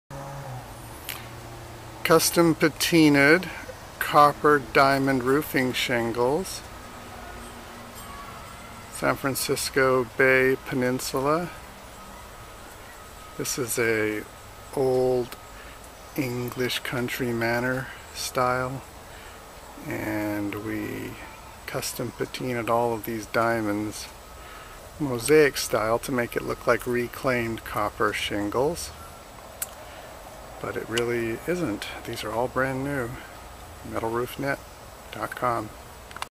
MRN H18 Pre-Patina Copper Diamonds
Our copper roofing diamond shingles were "Pre-Aged" in an irregular manner to make the resulting roof appear to be using re-claimed copper shingles.  Of course, they are brand new, but they DO look like they're already 100 years old.